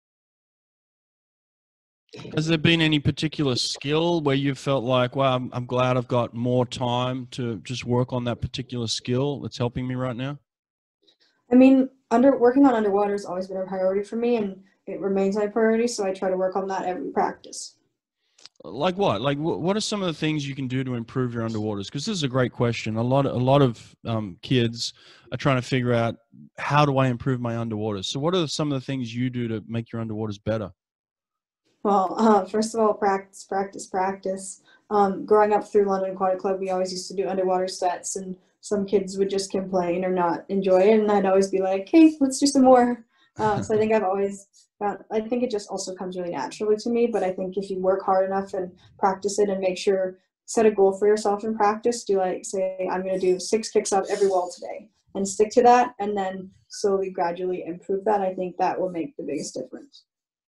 How do I improve my underwaters?
Practice, practice, practice. Every day practice.

Embrace underwater training.

Set daily swim practice goals.